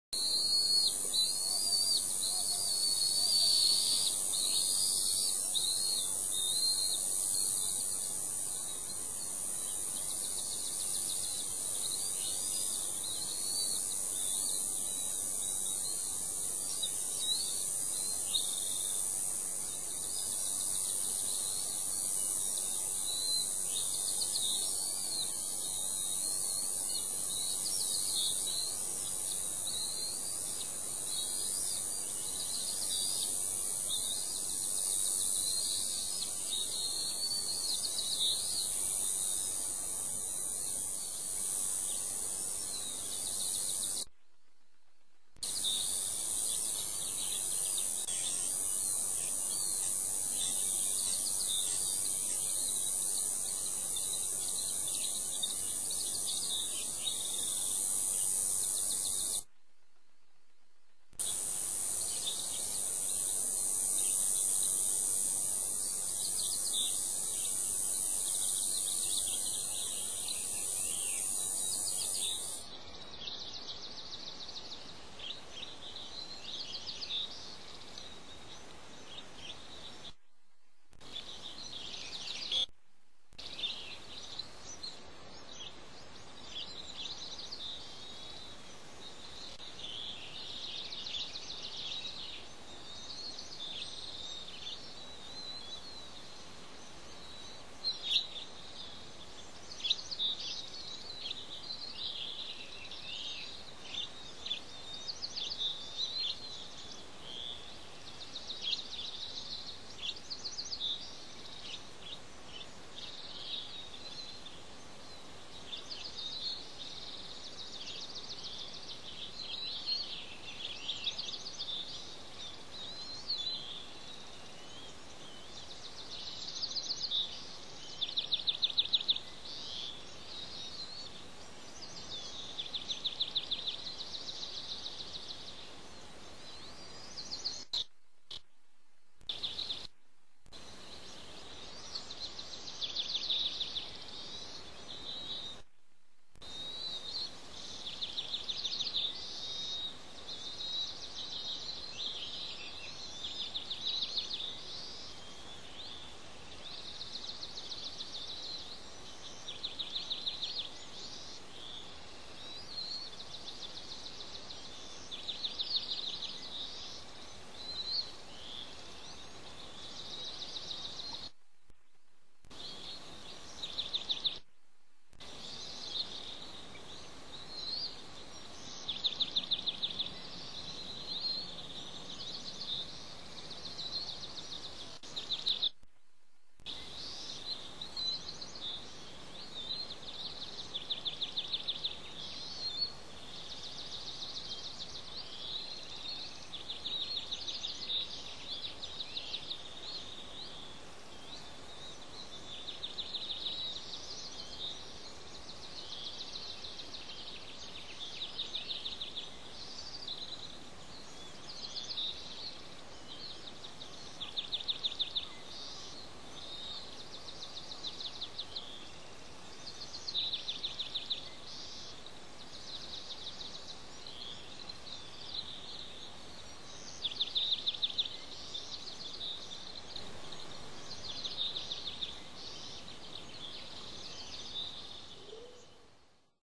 Dad's Quick Lunch With the Skye & Spirit
Dad (K10) brings a large fish to his Pelican Harbor nest on Santa Cruz Island for his two chicks (Skye & Spirit).  The older chick (Skye) grabs it first this time, but Dad takes it back a few seconds later and feeds Skye.  Doesn't look like Spirit got anything this time, though I could be wrong.

These eaglets are part of the ongoing restoration project by IWS.  You can read about the struggle to reintroduce America's symbol, the Bald Eagle, to the Channel Islands as well as watch this miracle of nature via live eagle-cam by going and clicking first on 'interactive' and then choosing one of the nests to view on Catalina Island and Santa Cruz Island or go